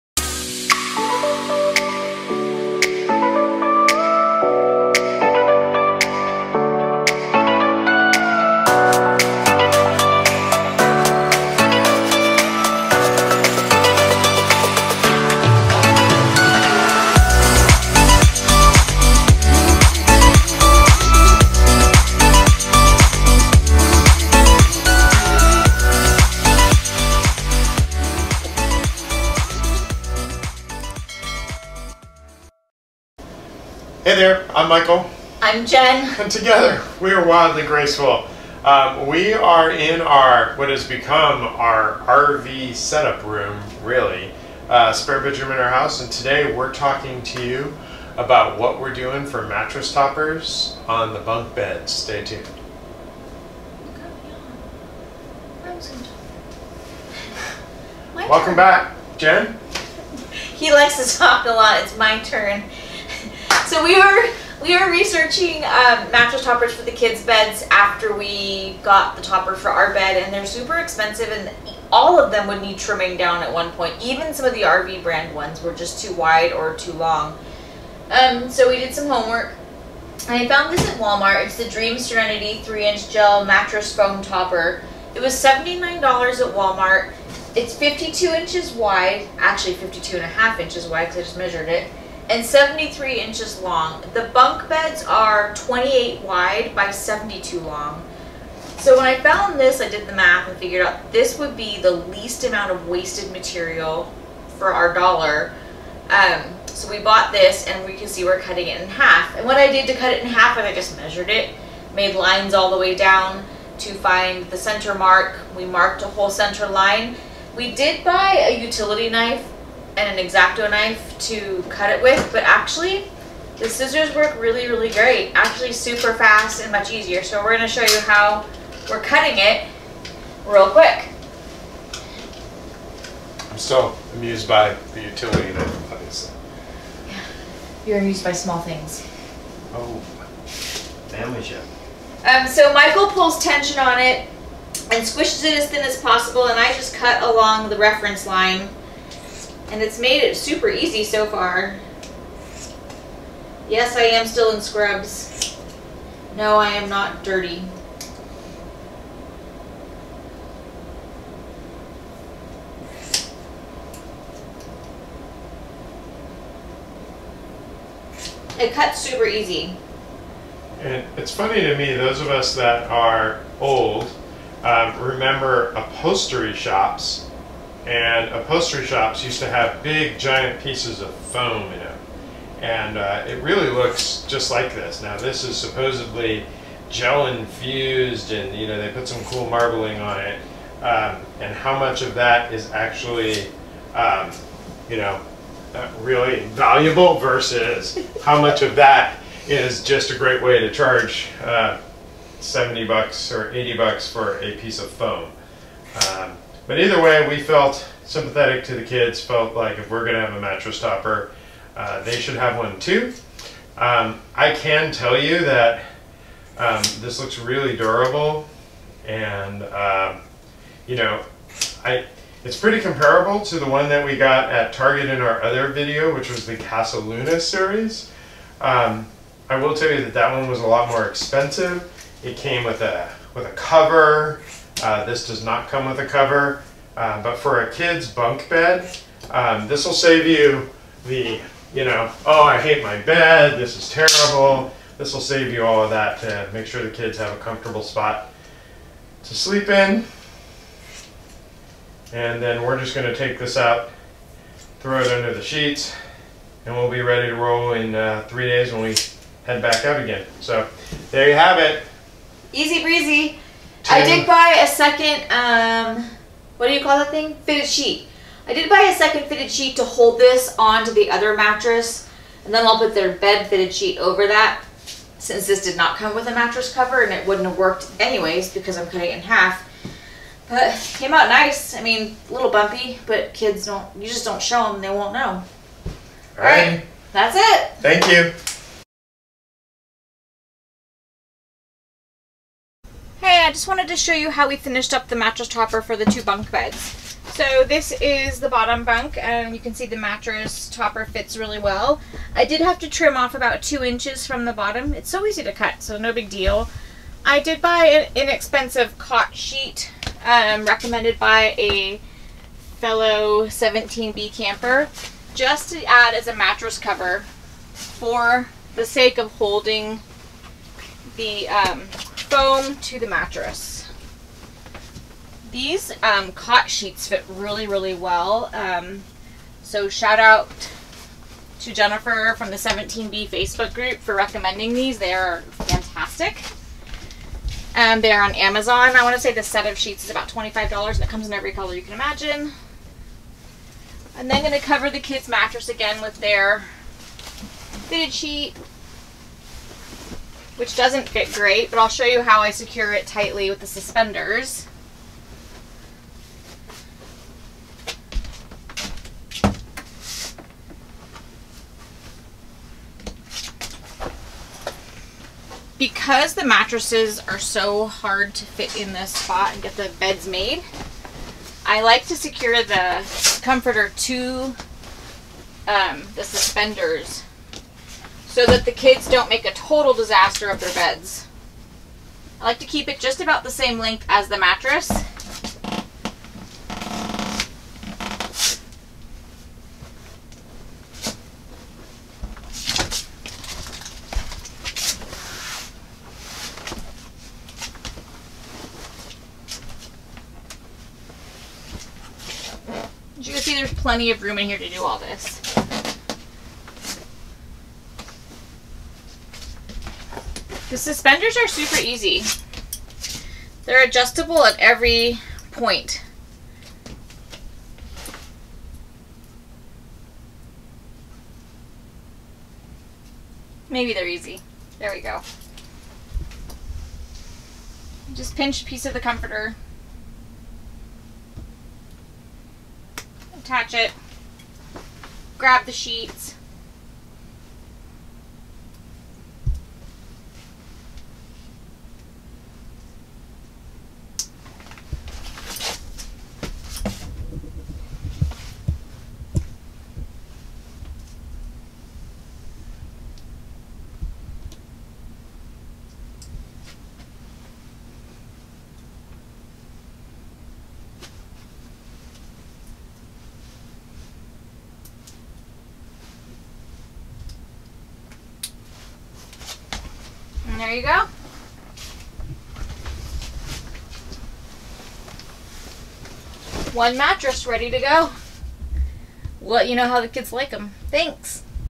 Kids Bunk Mattress Toppers
How do you top your trailer bunks?  Want to save money too?  These bunks are not Twin size nor are they comfortable for anyone to sleep on.  Left the bloopers in for entertainment!